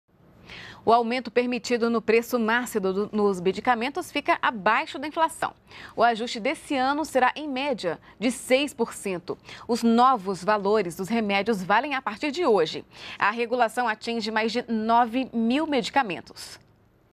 Medicamentos terão reajuste de no máximo 6% em 2015
NBR NOTÍCIAS - 31.03.15: A Câmara de Regulação do Mercado de Medicamentos (CMED) publicou no Diário Oficial da União desta terça-feira (31) o ajuste anual médio do preço máximo dos medicamentos para 2015. Fixado em 6%, o índice se manteve abaixo da inflação dos últimos 12 meses, que registrou 7,7%. O reajuste passa a valer a partir desta terça-feira.